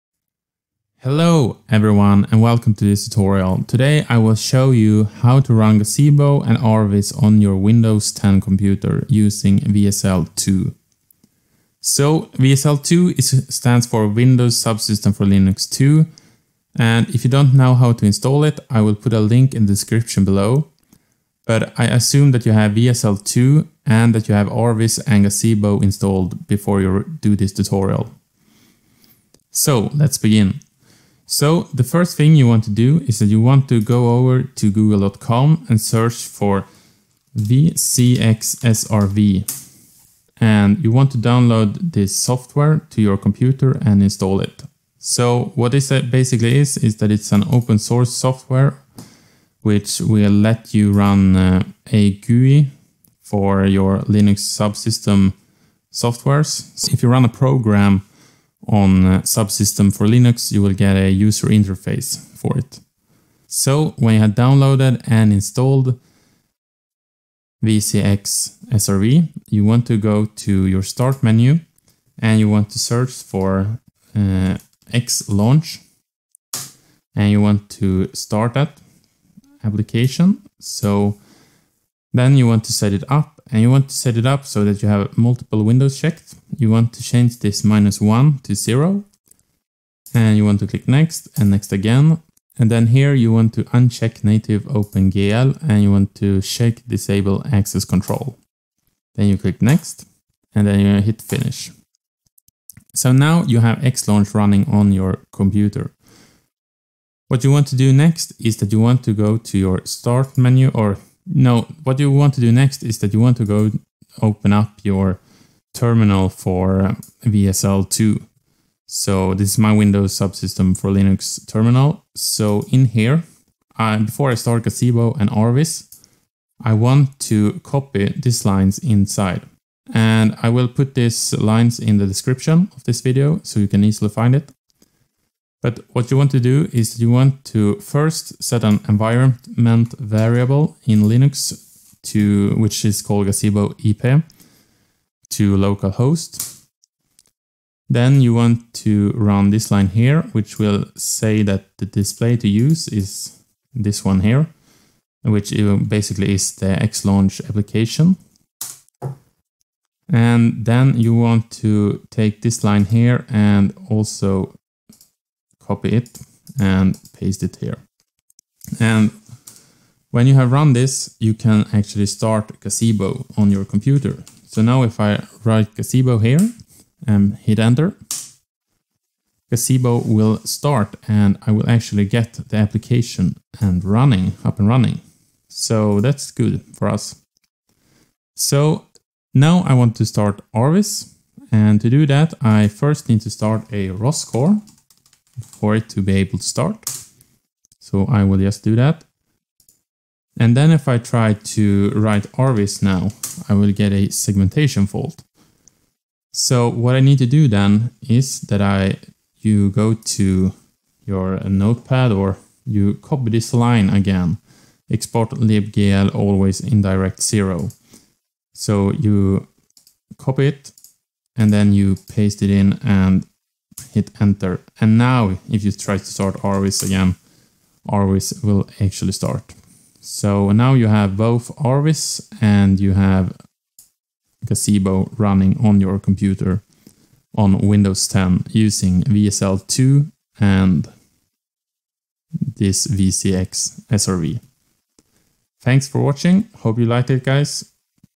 How to run Gazebo + RVIZ on Windows 10 using WSL2
In this video I will show you how to run Gazebo and RVIZ in Windows using WSL2. This is really helpful as it makes it opens up the posibility for ROS1 development on Windows 10.

Commands to run in WSL2:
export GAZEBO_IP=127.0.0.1
export DISPLAY=$(cat /etc/resolv.conf | grep nameserver | awk '{print $2}'):0
export LIBGL_ALWAYS_INDIRECT=0